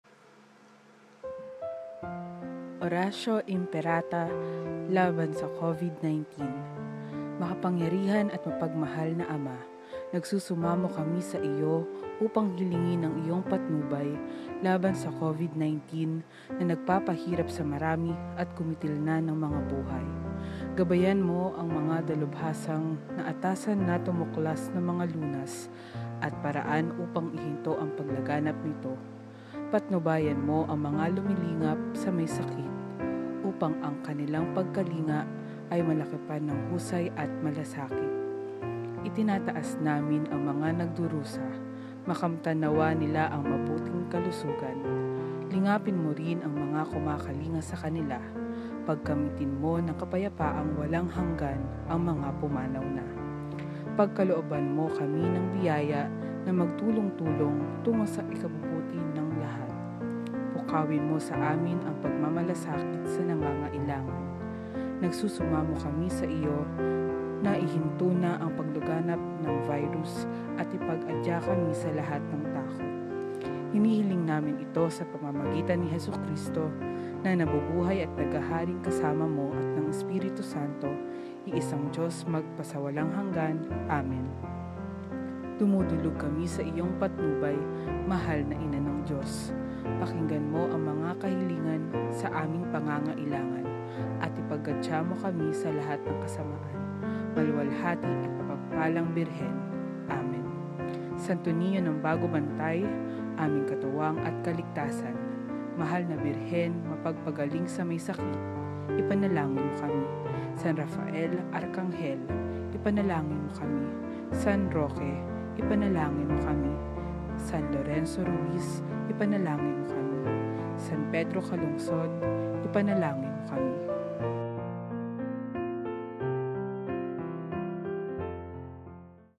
Oratio Imperata
Makapangyarihan at mapagmahal na Ama, nagsusumamo kami sa Iyo upang hilingin ang iyong patnubay laban sa COVID 19 na nagpapahirap sa marami at kumitil na ng mga buhay.

Gabayan Mo ang mga dalubhasang naatasan na tumuklas ng mga lunas at paraan upang ihinto ang paglaganap nito. Patnubayan Mo ang mga lumilingap sa maysakit upang ang kanilang pagkalinga ay malakipan ng husay at malasakit.

Itinataas namin ang mga nagdurusa. Makamtan nawa nila ang mabuting kalusugan. Lingapin mo rin ang mga kumakalinga sakanila. Pagkamitin Mo ng kapayapaang walang hanggan ang mga pumanaw na.

Pagkalooban mo kami ng biyaya na magtulong-tulong tungo sa ikabubuti ng lahat. Pukawin mo sa amin ang pagmamalasakit sa nangangailangan. Nagsusumamo kami na iyong ihinto na ang paglaganap ng virus at ipagadya kami sa lahat ng takot.

Hinihiling namin ito sa pamamagitan ni Hesukrito na nabubuhay at naghaharing kasama Mo at ng Espiritu Santo, iisang Diyos magpasawalang hanggan. Amen

Dumudulog kami sa Iyong patnubay, Mahal na Ina ng Diyos. Pakinggan mo ang mga kahilingan sa aming pangangailangan at ipagadya mo kami sa lahat ng kasamaan, maluwalhati at mapagpalang Birhen. Amen

Santo Niño ng Bago Bantay, aming katuwang at kaligtasan.

Mahal na Birhen, mapagpgaling sa maysakit, ipanalangin mo kami.

San Rafael Arkanghel, ipanalangin mo kami.

San Roque, ipanalangin mo kami.

San Lorenzo Ruiz, ipanalangin mo kami.

San Pedro Calungsod, ipanalangin mo kami.